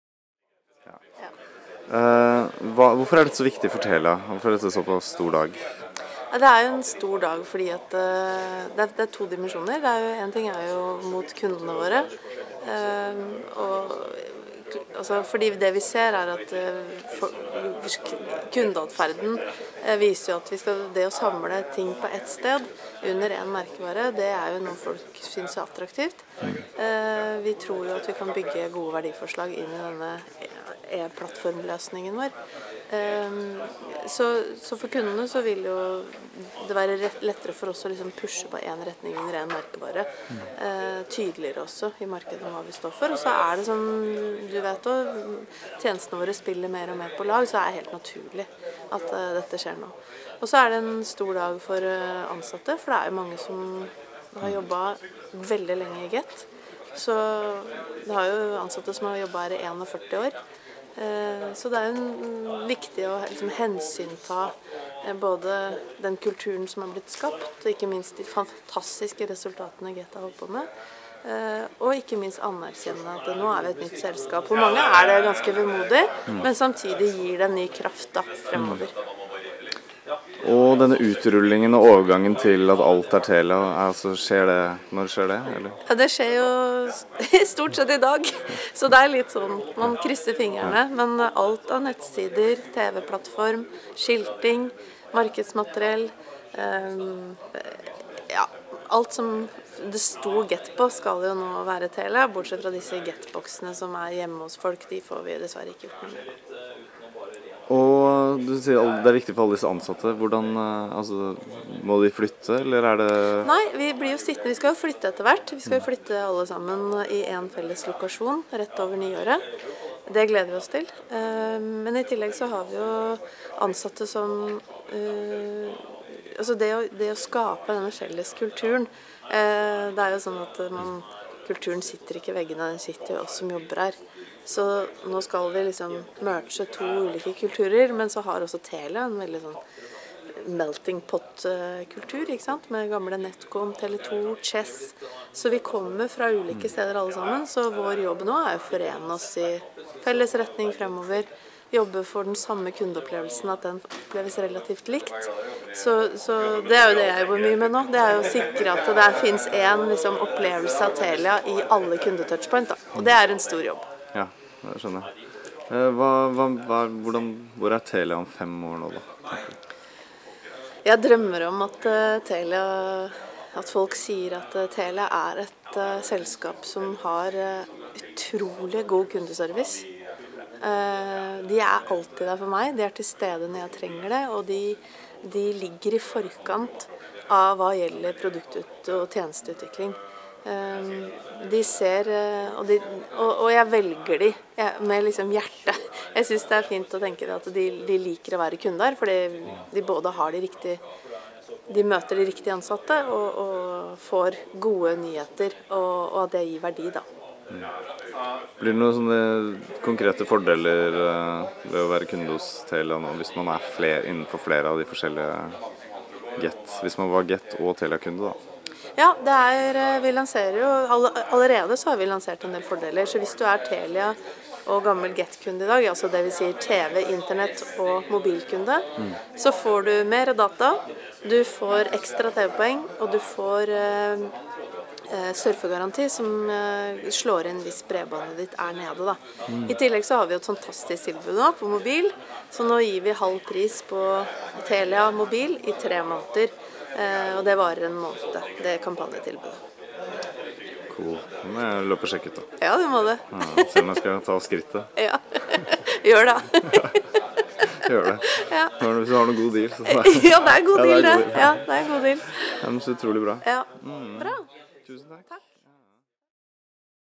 Get blir til Telia, men hvordan blir overgangen? Vi har intervjuet Camilla Forber.
Intervju med Camilla Forber, Markedsdirektør i Telia Norge.
Tirsdag 15. september i Telia Norges hovedkontor i Nydalen.